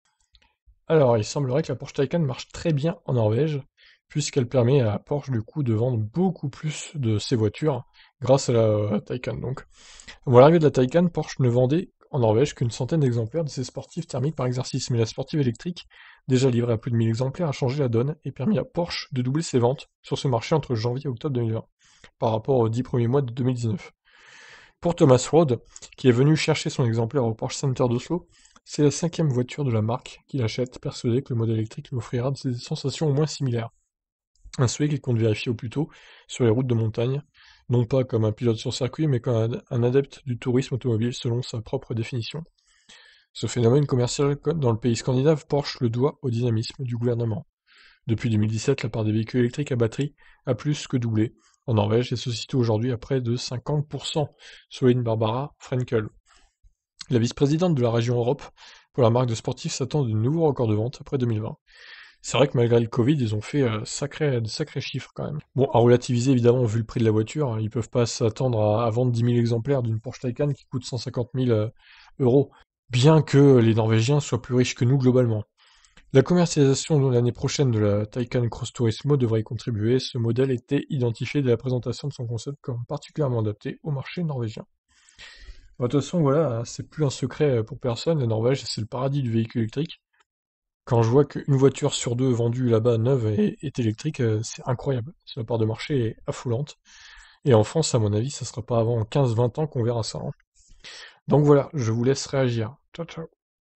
La Porsche Taycan plait bien aux Norvégiens
Et c'est Porsche qui tire son épingle du jeu avec des chiffres de ventes plus que doublés !